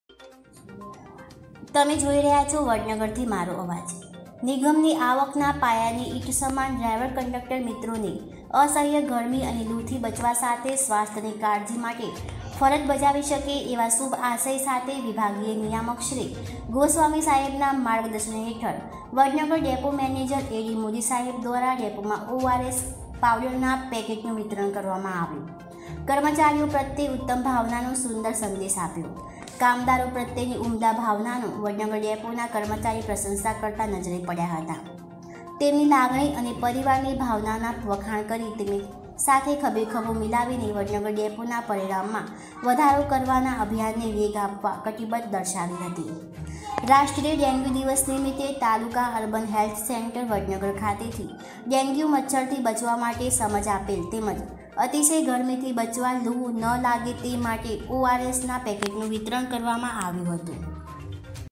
વડનગર એસટી ડેપોમાં ડ્રાઈવર/કંડકટરને ડેન્ગ્યુ મચ્છર અને ગરમીથી બચવા માર્ગદર્શન અપાયું..
રાષ્ટ્રીય ડેન્ગ્યુ દિવસ નિમિત્તે તાલુકા અર્બન હેલ્થ સેન્ટર વડનગર ખાતેથી ડેન્ગ્યુ મચ્છરથી બચવા માટે સમજ આપેલ તેમ જ અતિશય ગરમીથી બચવા અને લુ ન લાગે તે માટે ORS ના પડીકા નું વિતરણ કરવામાં આવેલ🙏